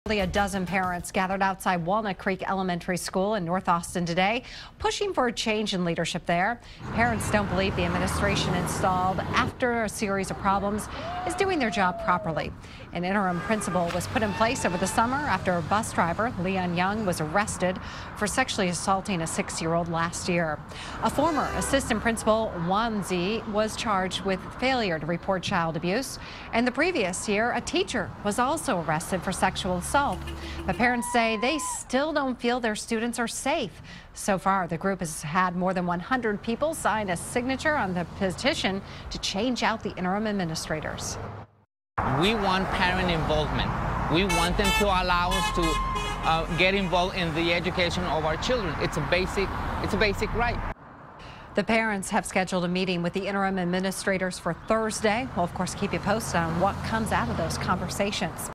Parents gathered outside Walnut Creek pushing for leadership change
Don't believe administration is doing their job after two staff members charged with crimes